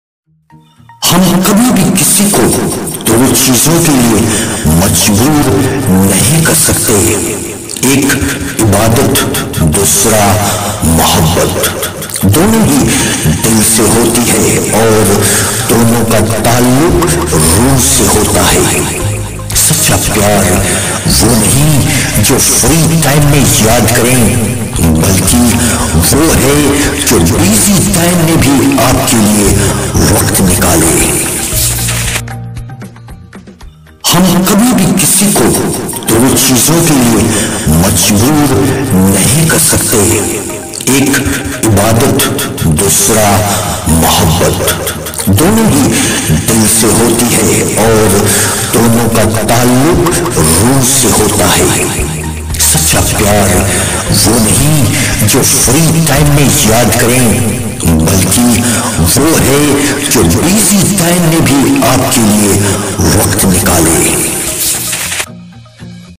MOTIVATIONAL THOUGHT SHORT VIDEO BY ASAF ALI.
Motivational thought short video on true love
by Asaf ali.
# voice over Asaf Ali.